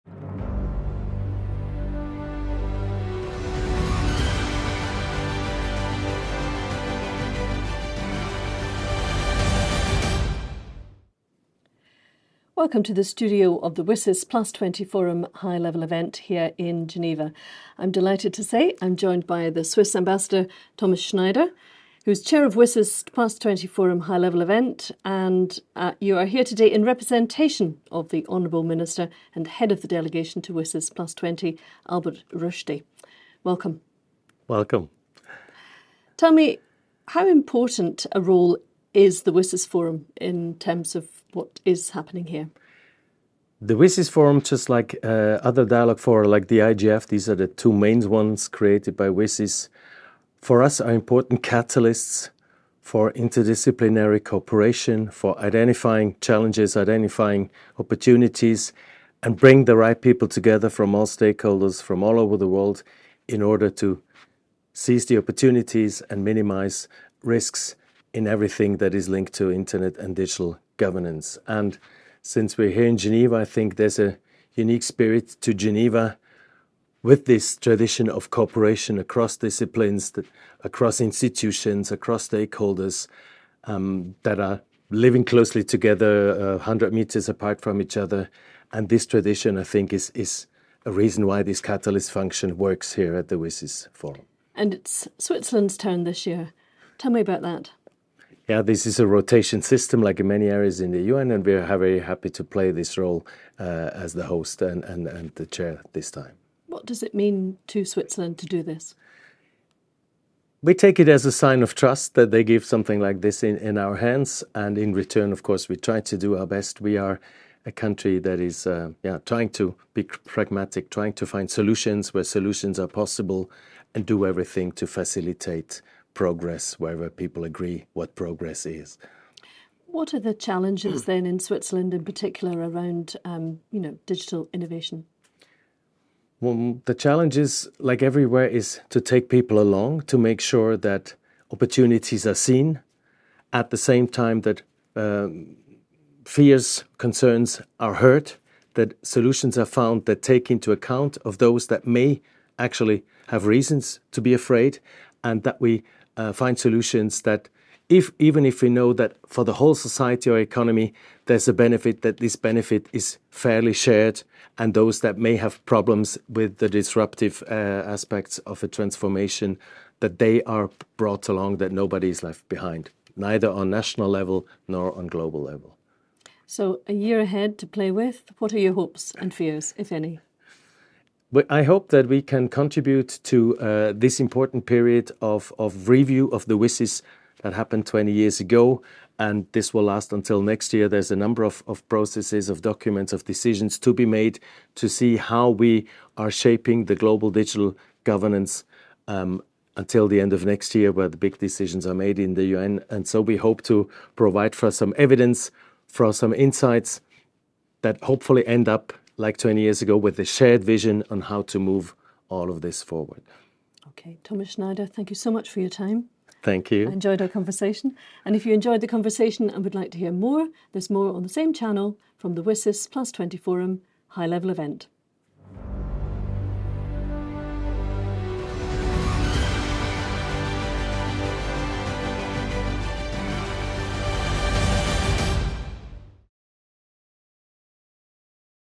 WSIS +20 FORUM HIGH-LEVEL EVENT 2024 INTERVIEW with Ambassador Thomas Schneider
Ambassador Thomas Schneider,  Chair of WSIS+20 Forum High-Level Event on behalf of Switzerland, in representation of H.E. Albert Rösti, Federal Councillor, Federal Office of Communications, Switzerland interviewed at WSIS +20 FORUM HIGH-LEVEL EVENT 2024 in Geneva, Switzerland.

The WSIS+20 Forum High-Level Event from 27 to 31 May 2024, commemorates two decades since the World Summit on the Information Society established a global framework for digital cooperation, aiming to build inclusive, people-centric information societies.